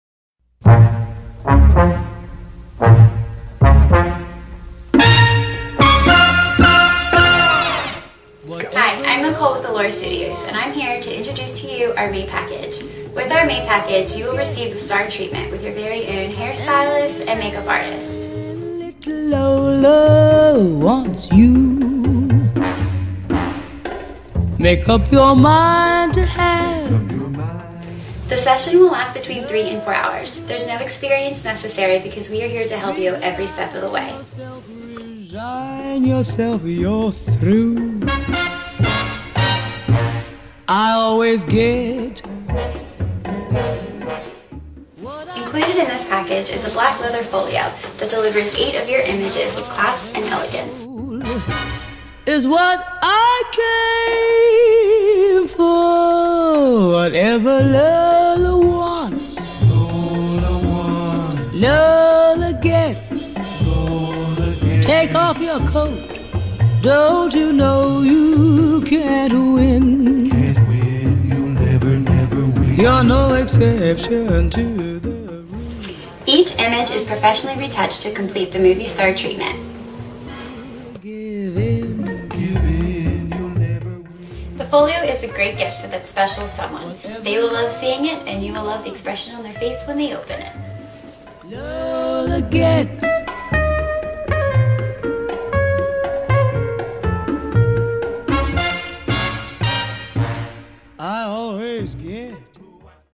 Allure Boudoir Photography - "Mae" Package
This video highlights the experience and products in the "Mae" package from Allure Boudoir Photography.  Allure Studios is located near Baltimore, Maryland in Harford County.  For more information about the package, visit our